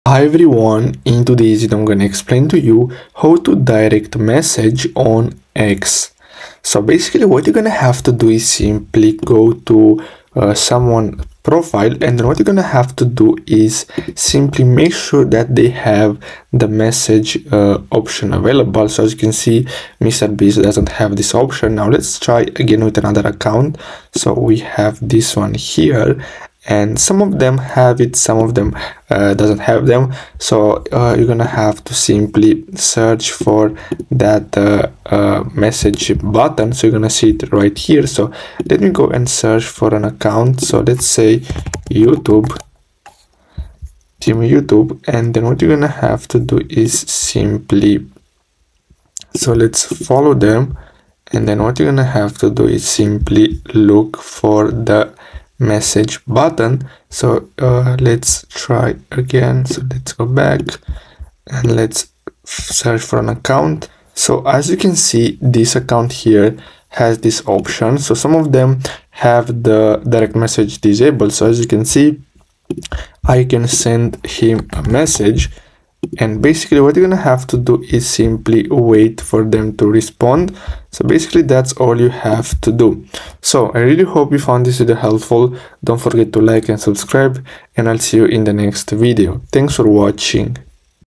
How To Direct Message On X (Twitter) - Full Guide
Learn how to make money for free Direct messaging on X Twitter is a convenient way to have private conversations with other users on the platform. To initiate a direct message, simply click on the envelope icon on the top right corner of your screen. From there, you can select the option to "compose new message" and type in the username of the person you wish to message.

You can also access your direct messages by clicking on the "messages" tab on the left side of your screen. It is important to note that direct messaging is only available between users who follow each other. Keep your messages professional and respectful to maintain a positive online presence.